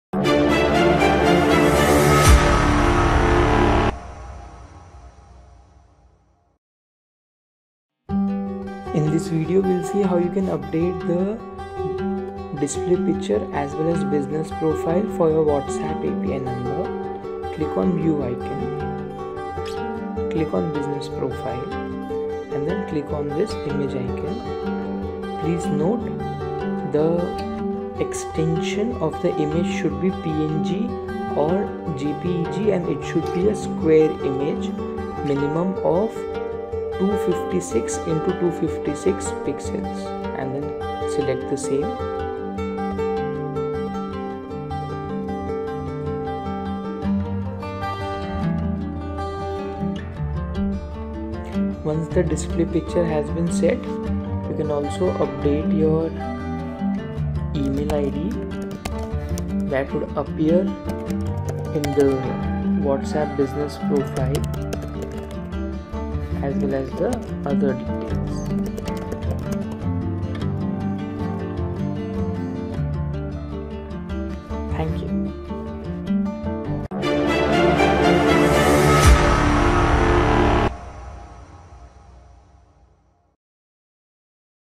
WhatsApp API: How To Update Business Profile And Display Picture Of Your Number? MageComp
In this Whatsapp API Tutorial, you will learn how you can update whatsapp business api profile and display your number.

By regularly updating your business profile and display picture on WhatsApp API, you can create a positive and lasting impression on your customers, foster trust, and build a strong brand identity in the digital marketplace.